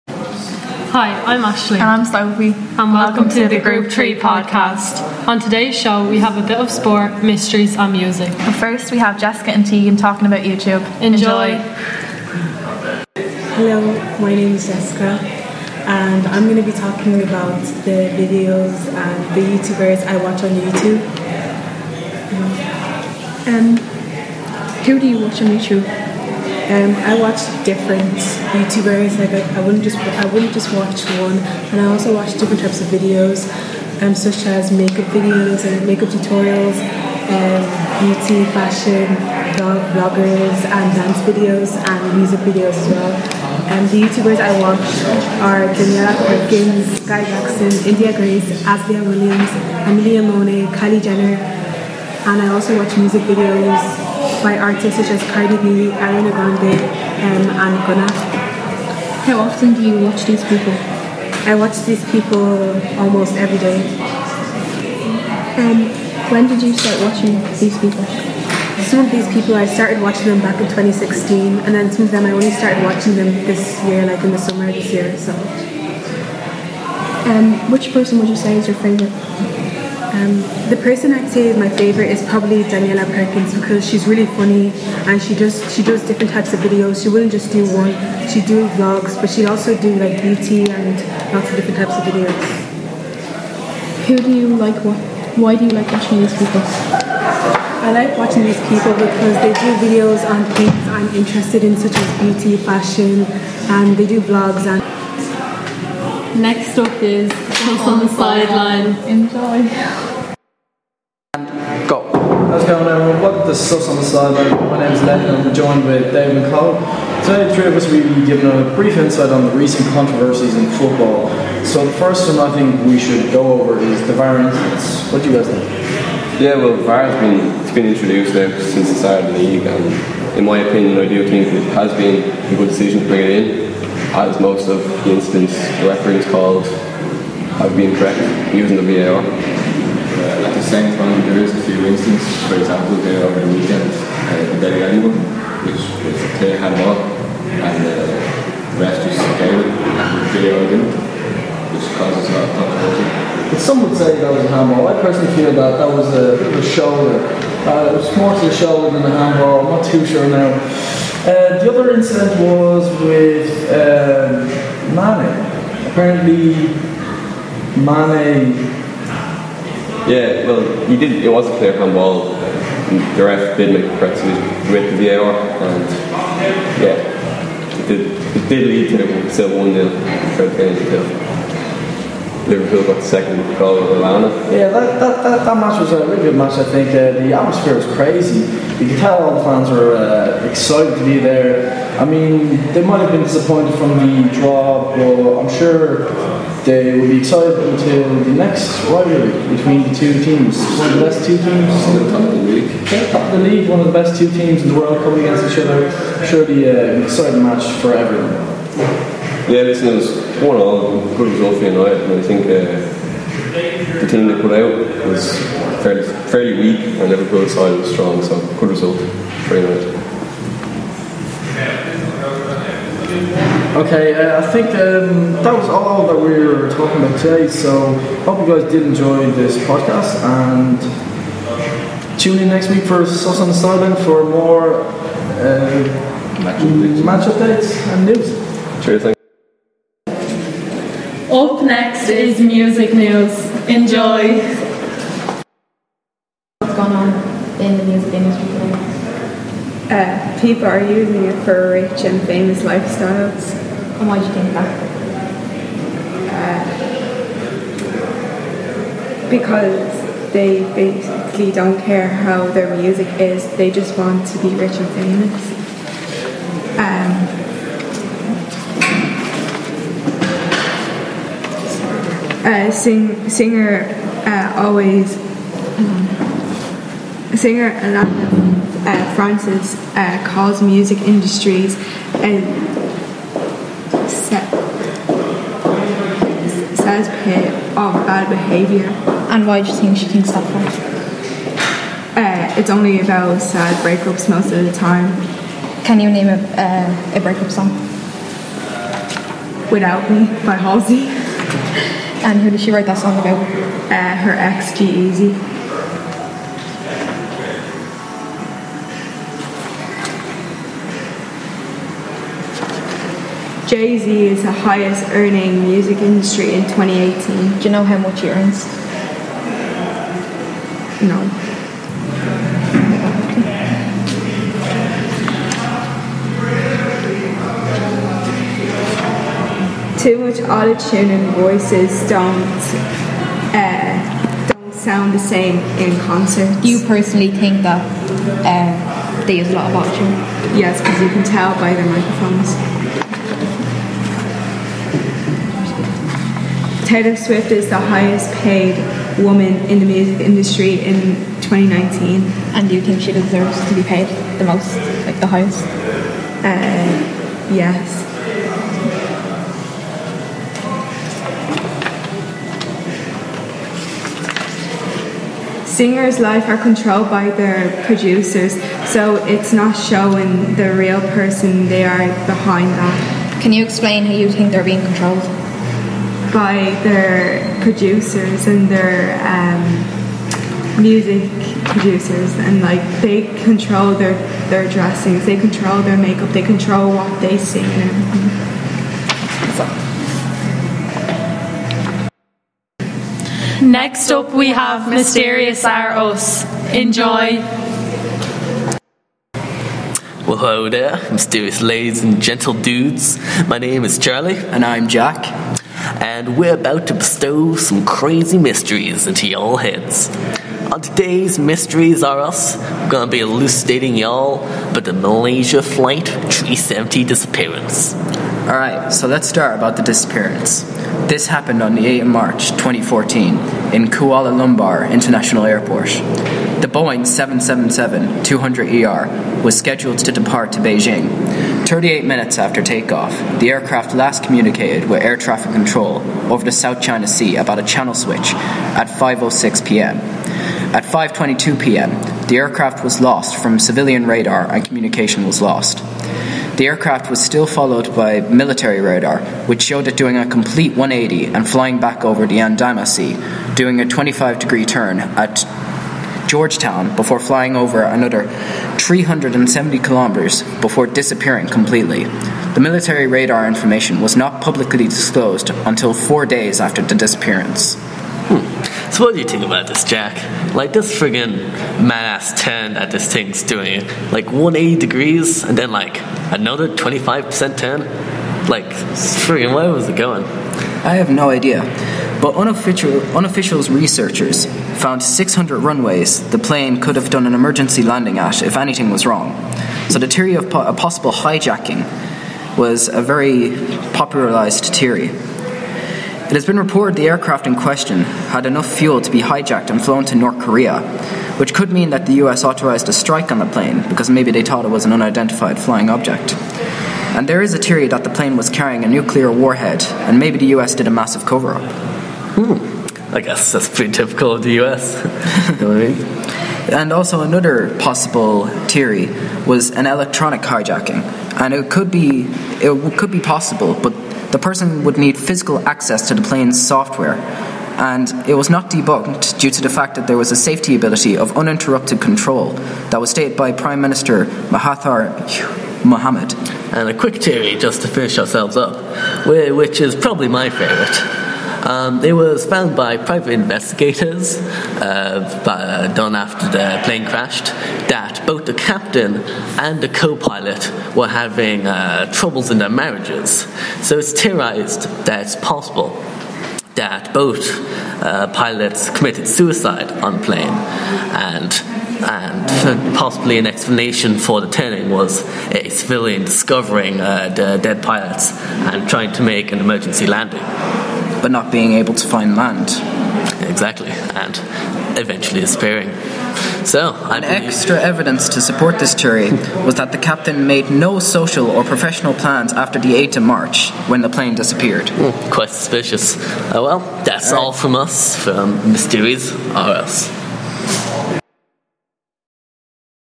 Createschool Radio featuring Lucan Community College Day1 ( Ep 1)
First group on Day 1 of podcasts at Lucan Community School